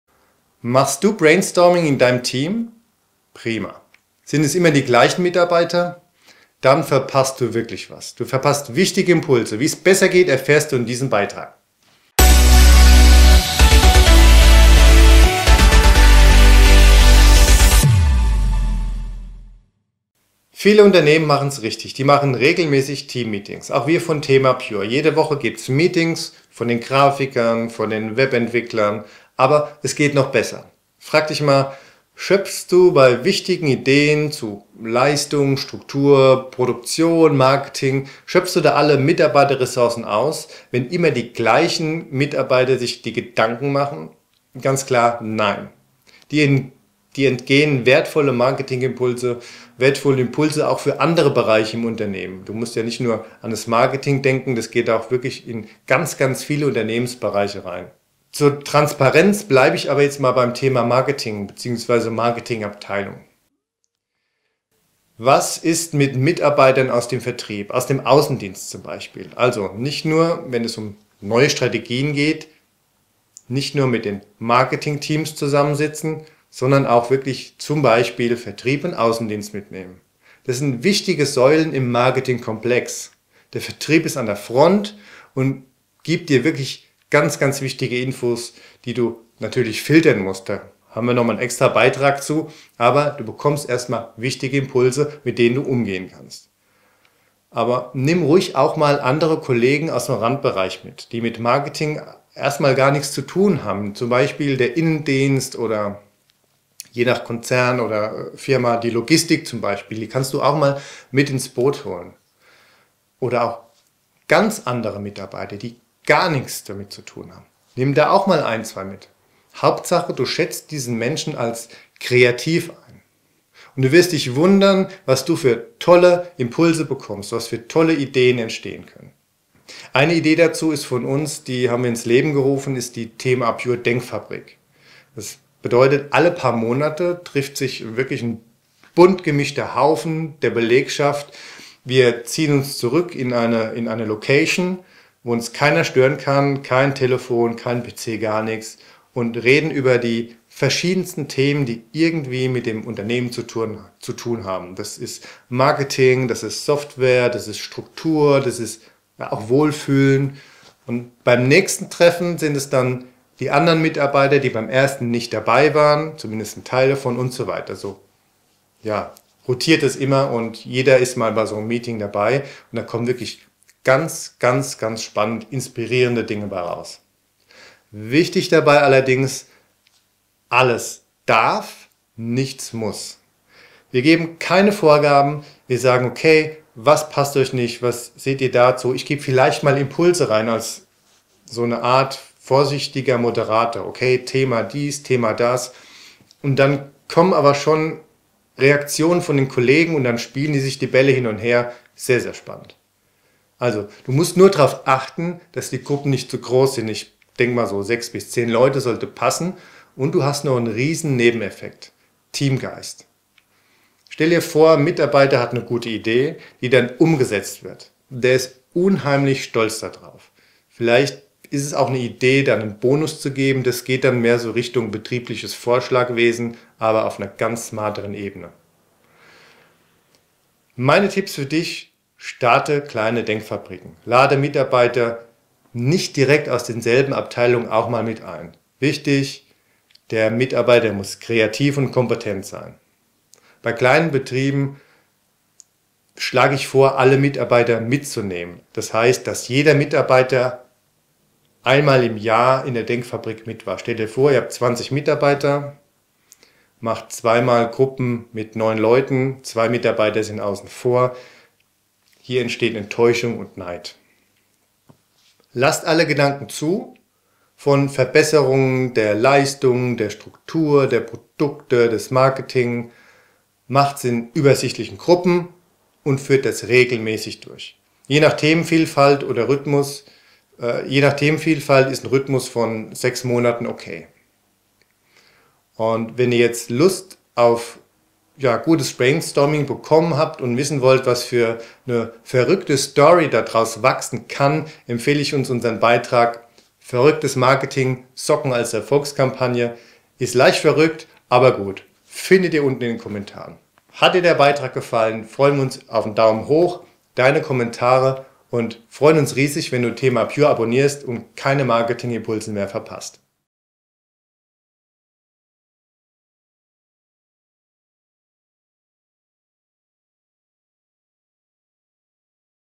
Vertane Chance - Brainstorming im Team | tma pure Marketing Podcast
Machst du Brainstorming mit deinem Team? Prima! Sind immer dieselben Mitarbeiter dabei? Wenn ja, verpasst du wirklich etwas …

Wenn dir das Video gefallen hat, freue ich mich auf deinen 👍 und deinen Kommentar. Teile das Video gerne auch mit deinen Kollegen, Freunden und Bekannten!

Viele Unternehmen gehen den richtigen Weg und sorgen für regelmäßige Team-Meetings, das ist bei uns z. B. das Team aus der Grafikabteilung. Jede Woche findet da ein reger Informationsaustausch zu Projekten, Software, Ideen etc. statt.

Doch es geht noch besser! Schöpfst du bei gewinnbringenden Ideen zu Leistungen, Struktur, Produkten oder Marketing alle Ressourcen aus, wenn sich immer dieselben Mitarbeiter darüber Gedanken machen? Ganz klar: NEIN – dir entgehen wertvolle Impulse für dein Marketing oder auch für andere Bereiche in deinem Unternehmen. Das muss sich nicht nur auf den Bereich Marketing beziehen. Das kannst du auf ganz viele Unternehmensbereiche übertragen.

Wie richtig gutes Brainstorming funktioniert, erzähle ich dir in unserem Video „Die vertane Chance – Brainstorming im Team“. Also los geht’s: klicken, ansehen, kommentieren – und mit neuen Impulsen durchstarten.